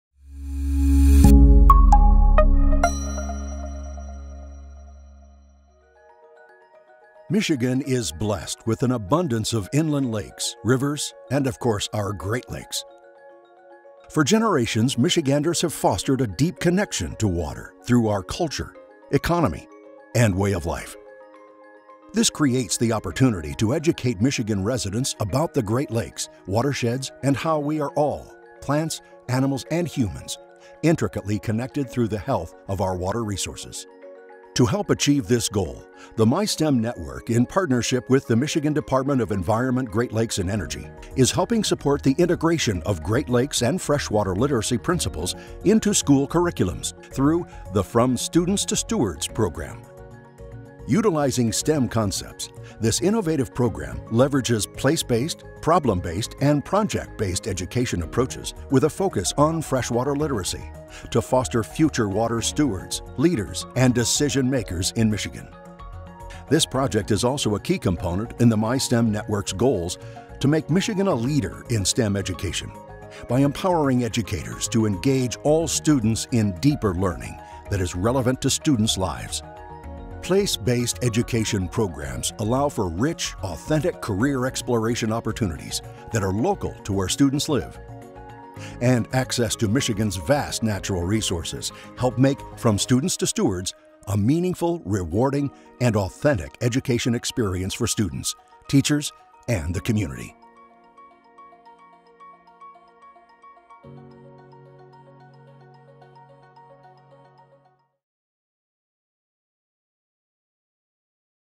Great Lakes and Fresh Water Week - Educate
From generation to generation, parents educate their children about Michigan’s lakes and streams. Don’t pollute. Circle of life. Respect. Stewardship. For students across the state, it’s a hands-on lesson as basic as learning reading, writing and arithmetic. Celebrate Great Lakes and Fresh Water Week by taking a moment to EDUCATE yourself and others on our fresh water resources.